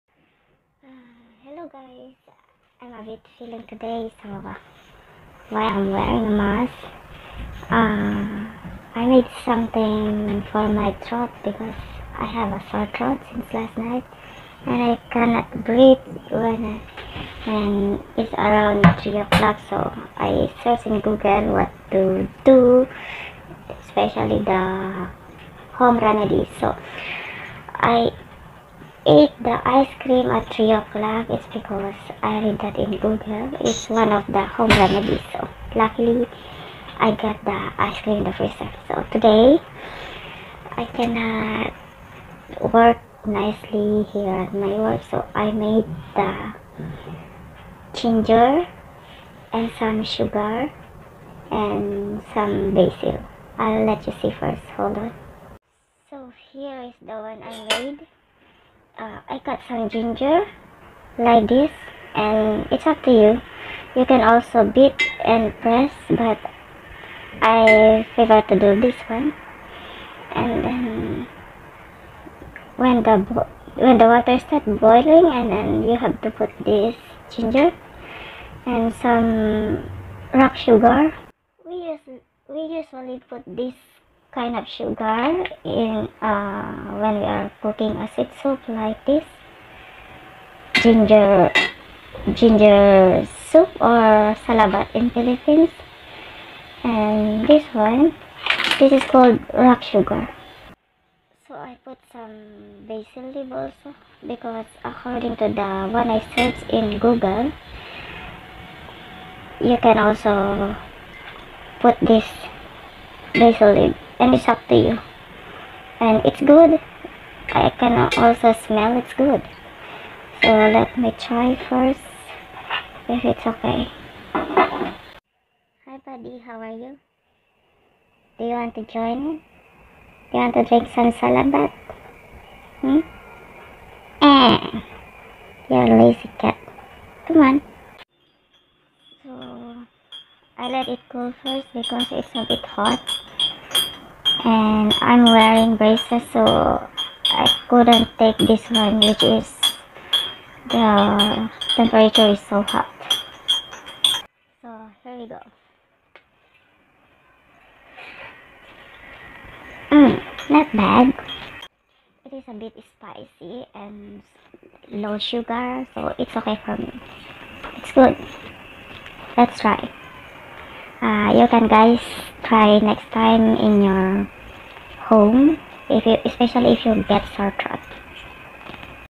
sorethroatremedies ginger basil
I just tried the home remedies in my work which is i search it in google.you can guys watch it maybe it can help if you try at home.